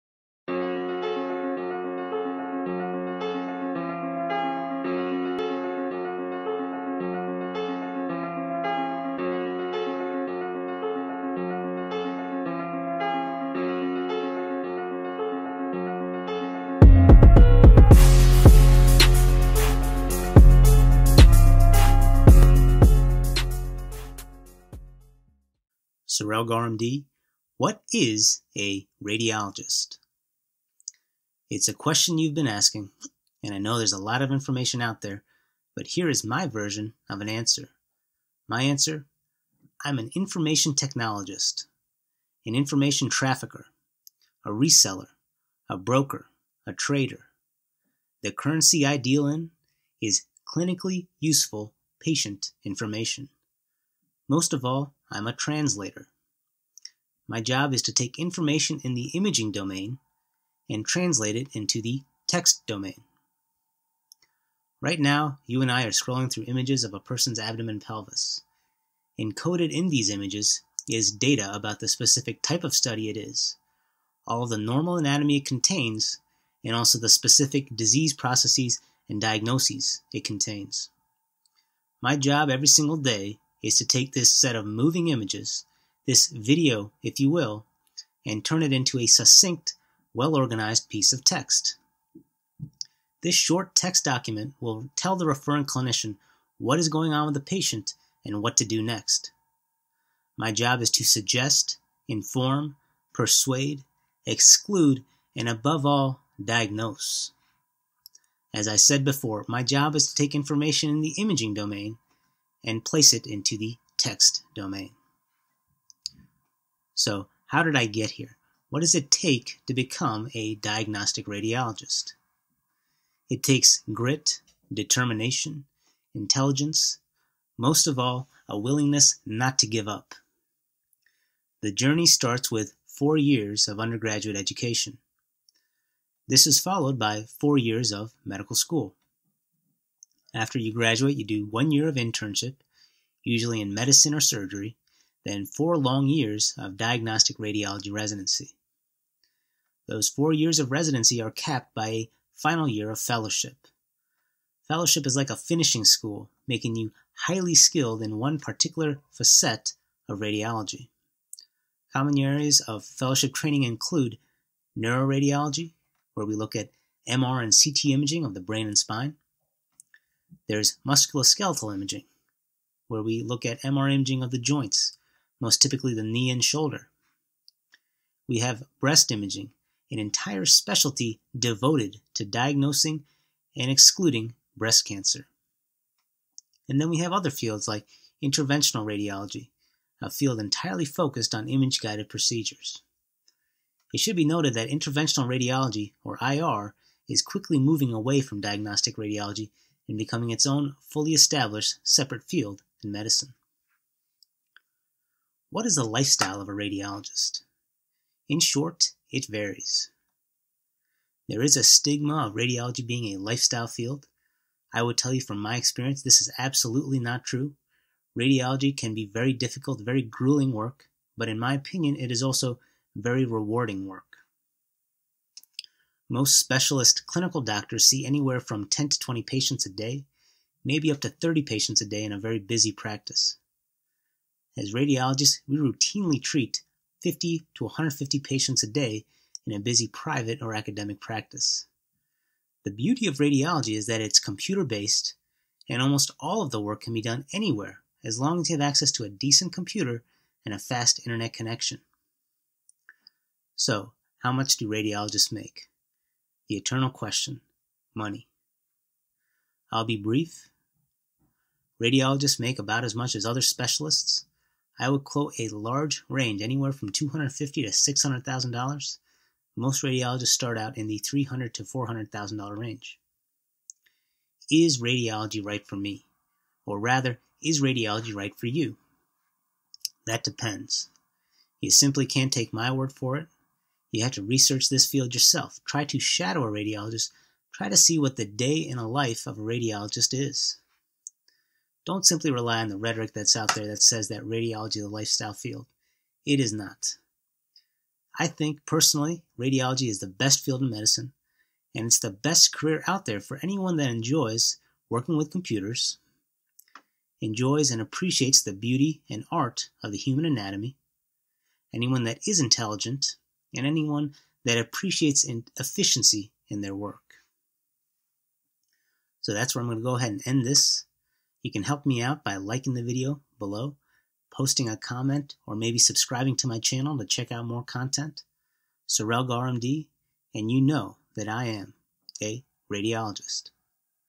What is a Radiologist?
SARELGAURMD discusses what a radiologist is, what it takes to become a radiologist, what kind of money they earn, and why you should become a radiologist.